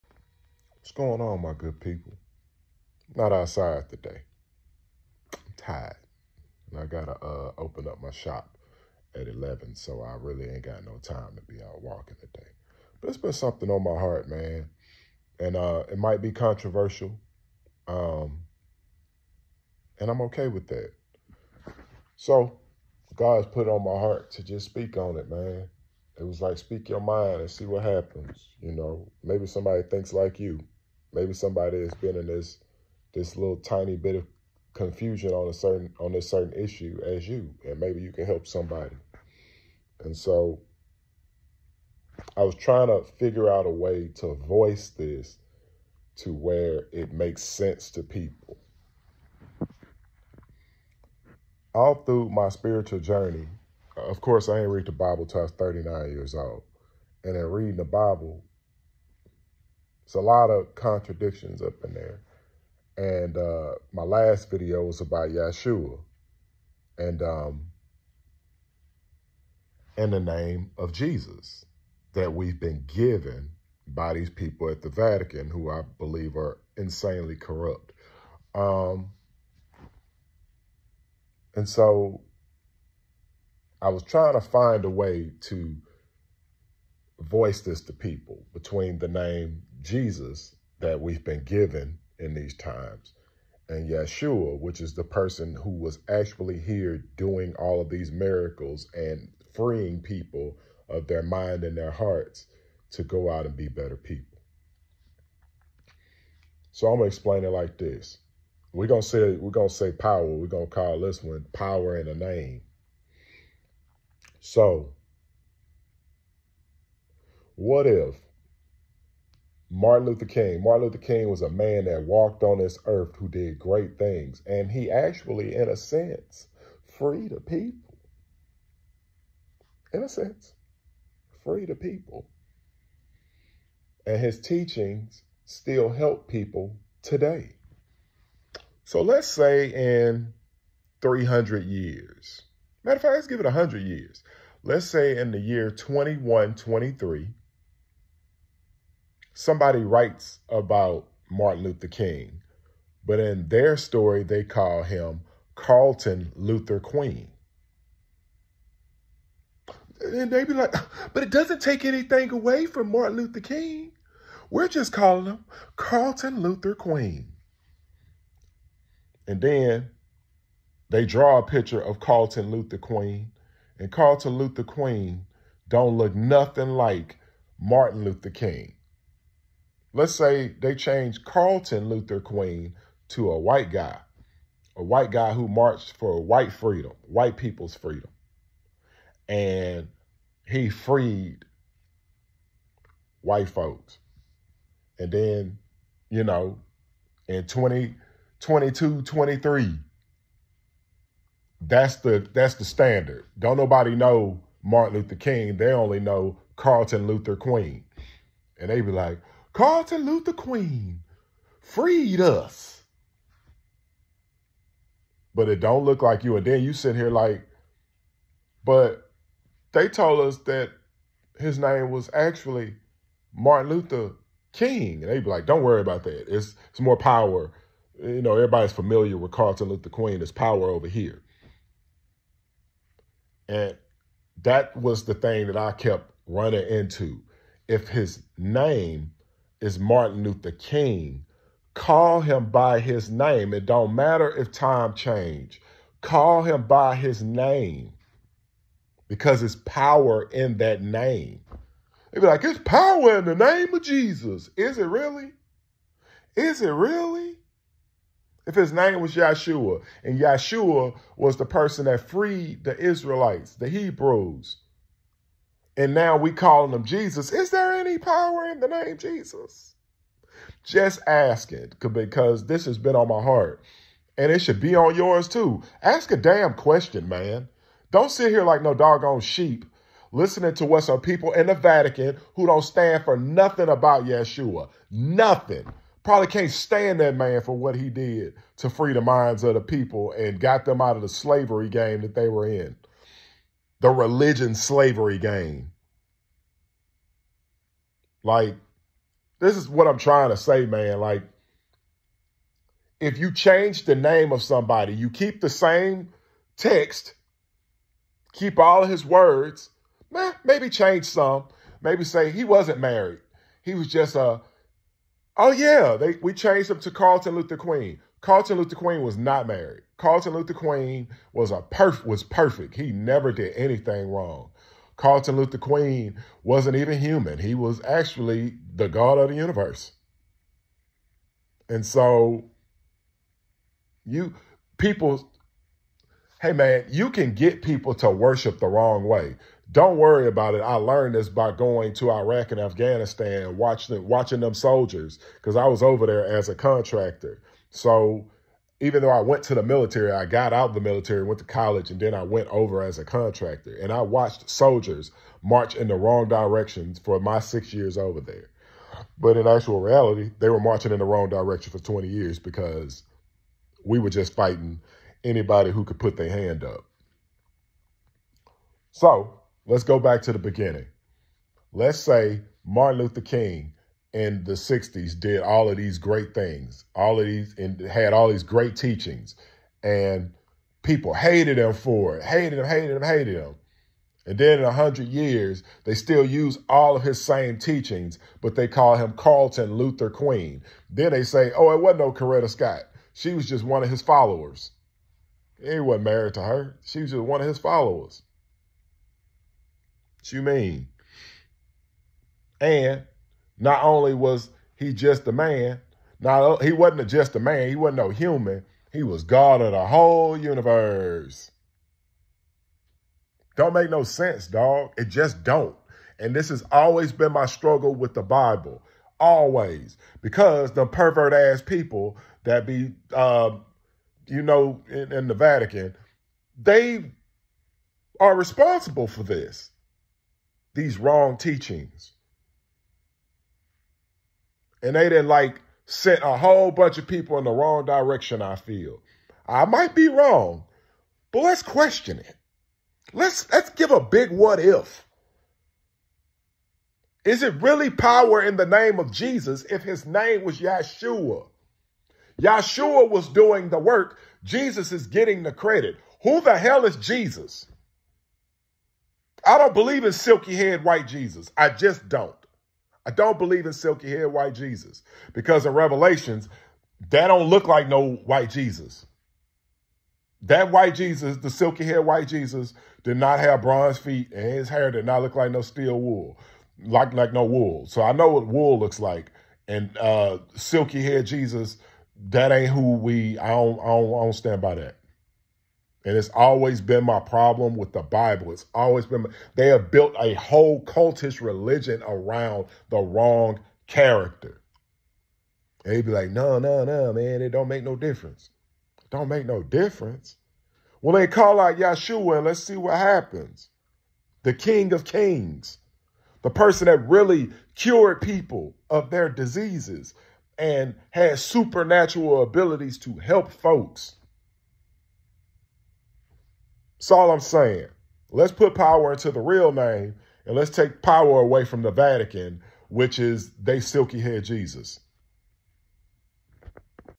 Is it really power in the name “Jesus”
In this video I tell a story of Yeshua and how the power has been given to someone else…